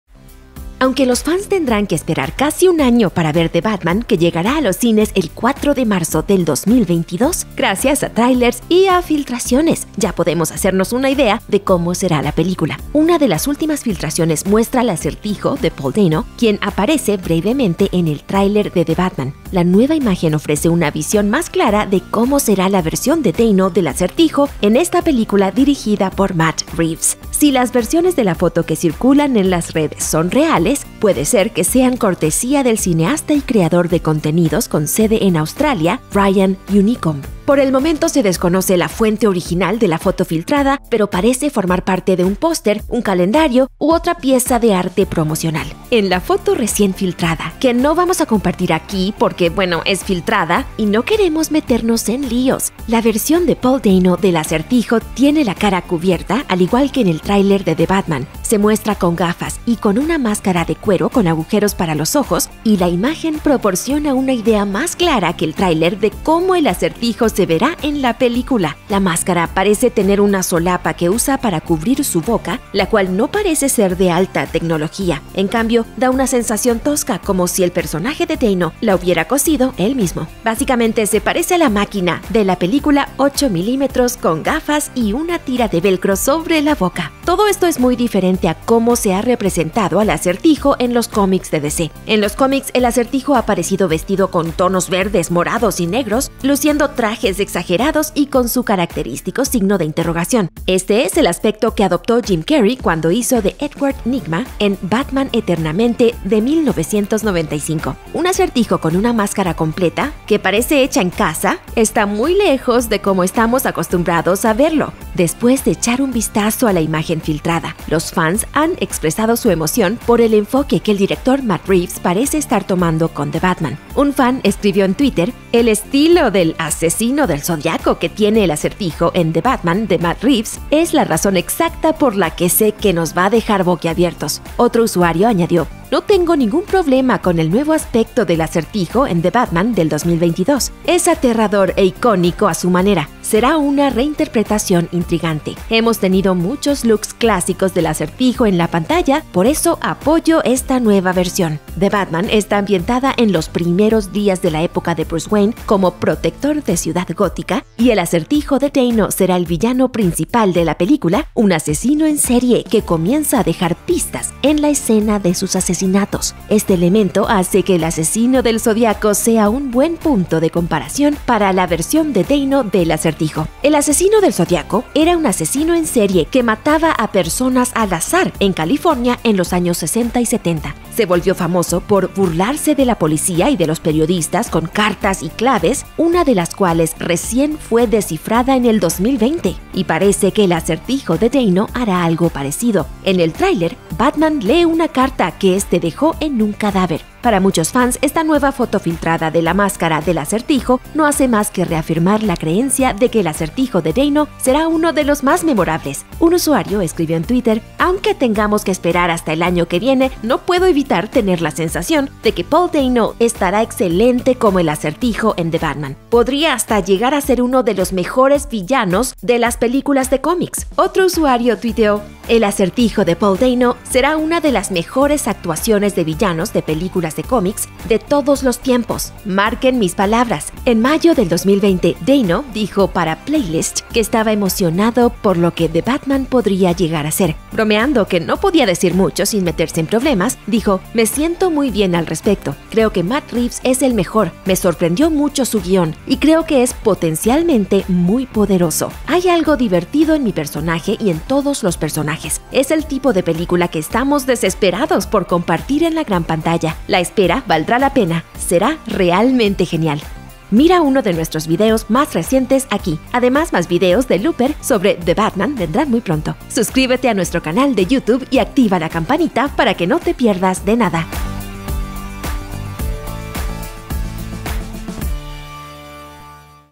Los Fans Quedaron Atónitos Al Ver La Imagen Filtrada Del Acertijo
Aunque los fans tendrán que esperar casi un año para ver The Batman -que llegará a los cines el 4 de marzo del 2022-, gracias a tráilers y a filtraciones, ya podemos hacernos una idea de cómo será la película. Una de las últimas filtraciones muestra al Acertijo de Paul Dano, quien aparece brevemente en el tráiler de The Batman.

La nueva imagen ofrece una visión más clara de cómo será la versión de Dano del Acertijo en esta película dirigida por Matt Reeves. Si las versiones de la foto que circulan en las redes son reales, puede ser que sean cortesía del cineasta y creador de contenidos con sede en Australia Ryan Unicomb. Por el momento se desconoce la fuente original de la foto filtrada, pero parece formar parte de un póster, un calendario u otra pieza de arte promocional.

¡En la foto recién filtrada -que no vamos a compartir aquí porque, bueno, es filtrada y no queremos meternos en líos! - la versión de Paul Dano del Acertijo tiene la cara cubierta, al igual que en el tráiler de The Batman. Se muestra con gafas y con una máscara de cuero con agujeros para los ojos, y la imagen proporciona una idea más clara que el trailer de cómo el Acertijo se verá en la película. La máscara parece tener una solapa que usa para cubrir su boca, la cual no parece ser de alta tecnología. En cambio, da una sensación tosca, como si el personaje de Dano la hubiera cosido él mismo. Básicamente, se parece a la Máquina de la película 8 mm con gafas y una tira de velcro sobre la boca.

Todo esto es muy diferente a cómo se ha representado al Acertijo en los cómics de DC. En los cómics, el Acertijo ha aparecido vestido con tonos verdes, morados y negros, luciendo trajes exagerados y con su característico signo de interrogación. Este es el aspecto que adoptó Jim Carrey cuando hizo de Edward Nygma en Batman Eternamente, de 1995. Un Acertijo con una máscara completa que parece hecha en casa está muy lejos de cómo estamos acostumbrados a verlo.

Después de echar un vistazo a la imagen filtrada, los fans han expresado su emoción por el enfoque que el director Matt Reeves parece estar tomando con The Batman. Un fan escribió en Twitter,

"El estilo del Asesino del Zodiaco que tiene el Acertijo en 'The Batman' de Matt Reeves es la razón exacta por la que sé que nos va a dejar boquiabiertos."

Otro usuario añadió,

"No tengo ningún problema con el nuevo aspecto del Acertijo en 'The Batman' del 2022. Es aterrador e icónico a su manera. Será una reinterpretación intrigante. Hemos tenido muchos looks clásicos del Acertijo en la pantalla, por eso apoyo esta nueva versión."

The Batman está ambientada en los primeros días de la época de Bruce Wayne como protector de Ciudad Gótica, y el Acertijo de Dano será el villano principal de la película, un asesino en serie que comienza a dejar pistas en la escena de sus asesinatos. Este elemento hace que el Asesino del Zodiaco sea un buen punto de comparación para la versión de Dano del Acertijo. El asesino del Zodiaco era un asesino en serie que mataba a personas al azar en California en los años 60 y 70. Se volvió famoso por burlarse de la policía y de los periodistas con cartas y claves, una de las cuales recién fue descifrada en el 2020. Y parece que el Acertijo de Dano hará algo parecido; en el tráiler, Batman lee una carta que éste dejó en un cadáver.

Para muchos fans, esta nueva foto filtrada de la máscara del Acertijo no hace más que reafirmar la creencia de que el Acertijo de Dano será uno de los más memorables.

Un usuario escribió en Twitter,

"Aunque tengamos que esperar hasta el año que viene, no puedo evitar tener la sensación de que Paul Dano estará excelente como el Acertijo en 'The Batman'. Podría hasta llegar a ser uno de los mejores villanos de las películas de cómics."

Otro usuario twitteó,

"El Acertijo de Paul Dano será una de las mejores actuaciones de villanos [de películas de cómics] de todos los tiempos, marquen mis palabras."

En mayo del 2020, Dano dijo para Playlist que estaba emocionado por lo que The Batman podría llegar a ser. Bromeando que no podía decir mucho sin meterse en problemas, dijo,

"Me siento muy bien al respecto. Creo que Matt Reeves es el mejor. Me sorprendió mucho su guión, y creo que es, potencialmente, muy poderoso. [...] Hay algo divertido en mi personaje y en todos los personajes. Es el tipo de película que estamos desesperados por compartir en la gran pantalla. [...] [La espera] valdrá la pena. Será realmente genial."